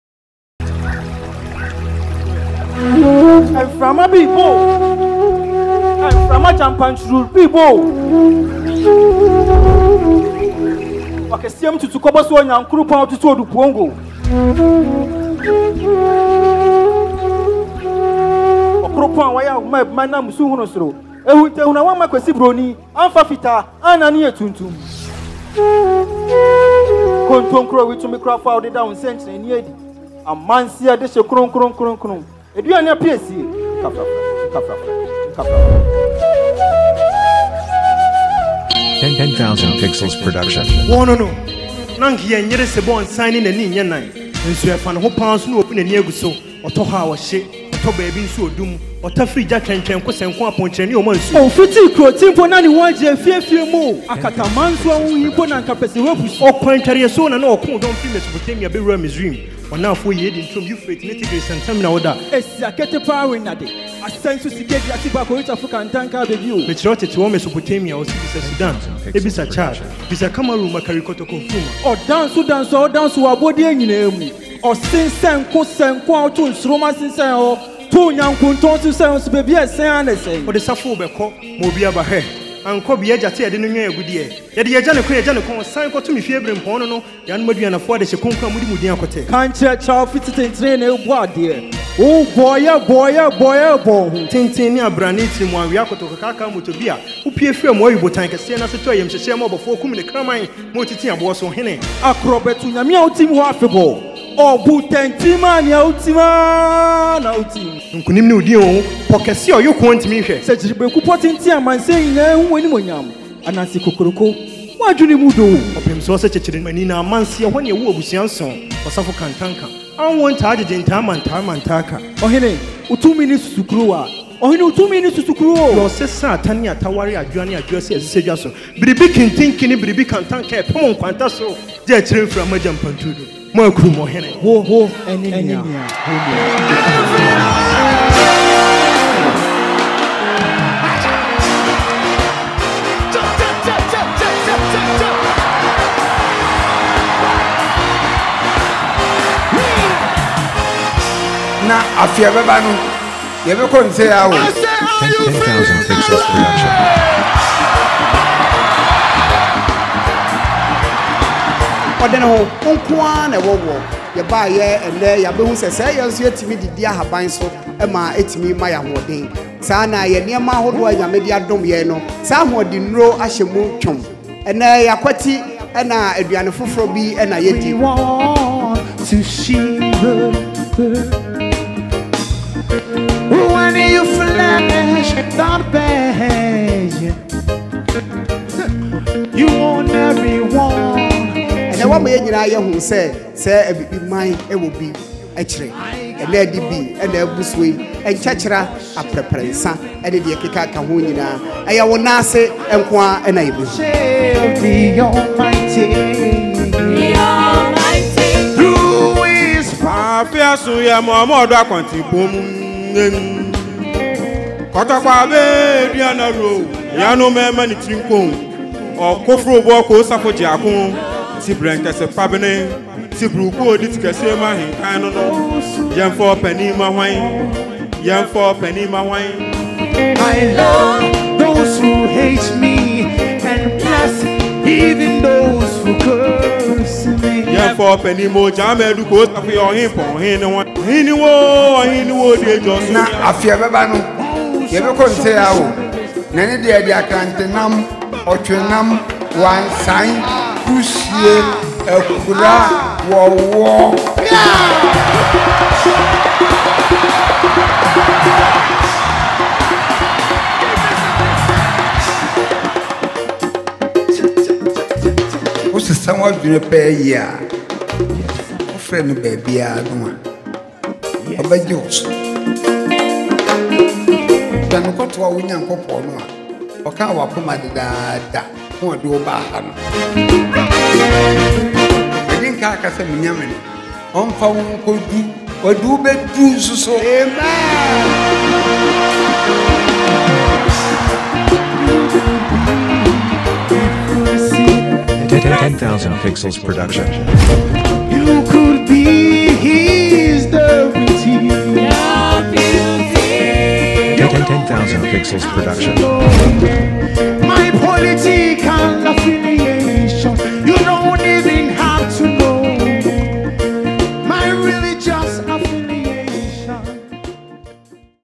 Apostle Dr Kwadwo Safo and Kristo Asafo celebrates unity and African tradition
Bringing you the best of the best...
Apostle Dr Ing. Safo celebrating the beauty of African tradition , Kristo Asafo Est. 1971 🌈 😍💫🌈Celebrating Africa beauty, food, praises, unity and more.. Must watch 🌟 The Star of Africa Shot & Directed Safo Jnr
Bookings open Uk, Belgium and Global
Videographer check @10thousandpixles

Congratulations  Apstl. Dr. Ing Kwadwo Safo 😍